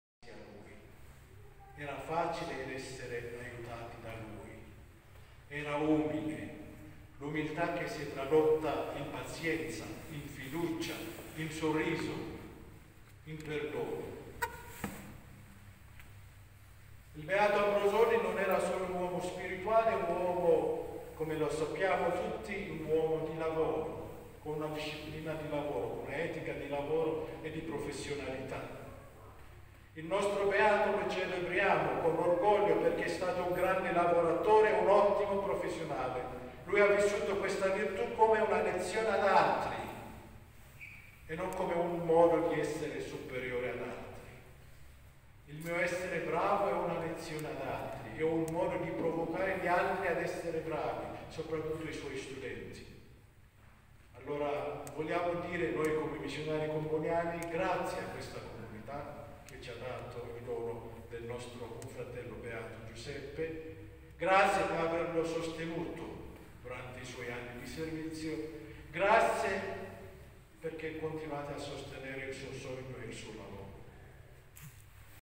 IL PADRE GENERALE DEI COMBONIANI A RONAGO:"UMILE E VICINO ALLA GENTE: ECCO CHI ERA PADRE GIUSEPPE"
Le parole di Gebresilasie Tadesse Tesfaye oggi pomeriggio alla chiesa parrocchiale di Ronago